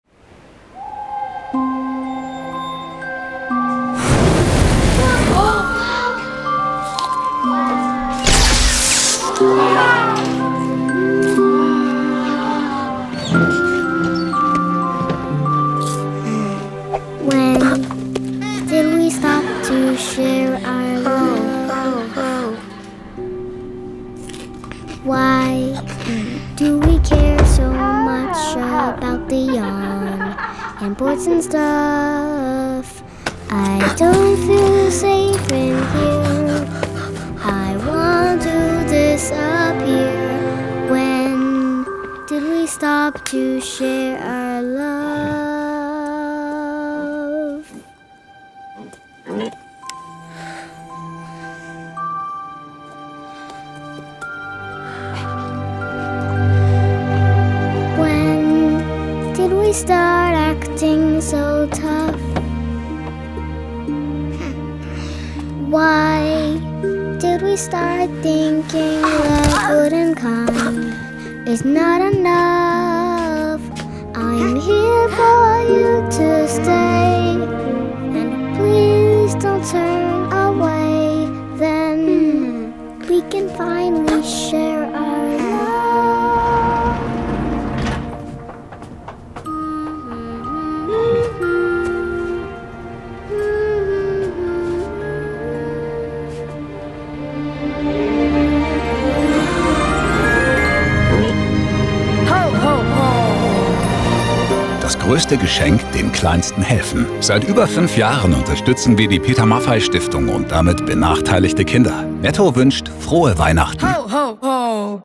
Die Geschichte vom wahren Weihnachtsmann I NETTO
Erlebe eine wahrhaft magische Weihnachtsgeschichte! DerWahreWeihnachtsmann zeigt uns, dass Liebe das schönste Geschenk ist – und dass gerade die Kleinsten das Größte verdienen. Netto wünscht ein frohes Fest!